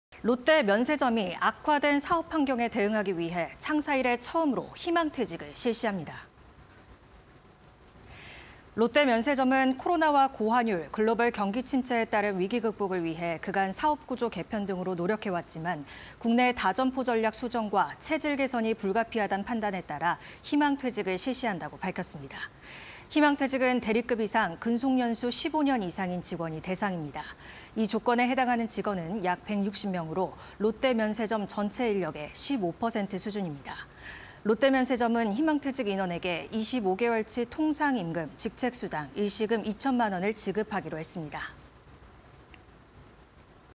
롯데면세점, 창사 이래 첫 희망퇴직 실시 (20221214)
롯데면세점, 창사 이래 첫 희망퇴직 실시